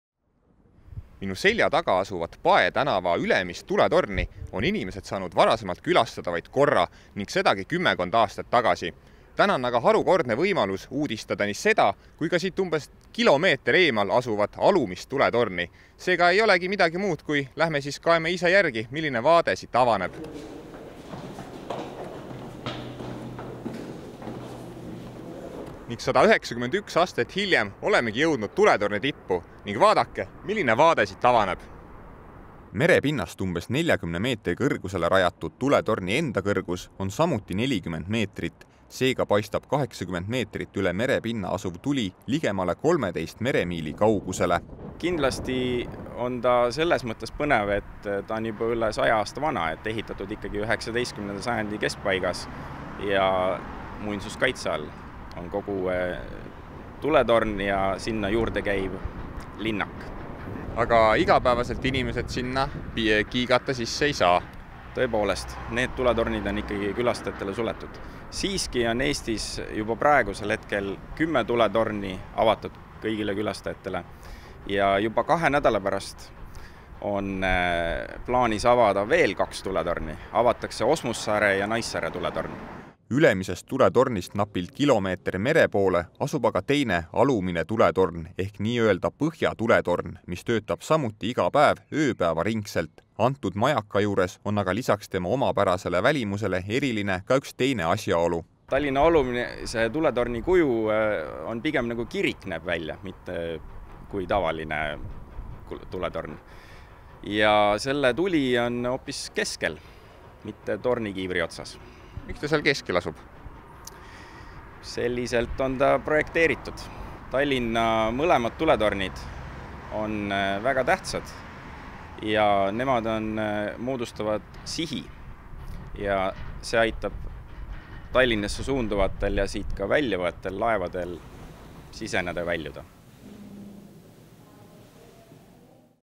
17.05.2019 - Tallinna kaks tuletorni avasid üheks päevaks oma uksed
Käimasoleva mereohutuskuu raames avas Veeteede Amet täna neljaks tunniks kõigile külastajatele kahe Tallinnas asuva tuletorni uksed. Huvilistel oli võimalus ronida nii tuletornide tippu kui ka tutvuda nende ajalooga.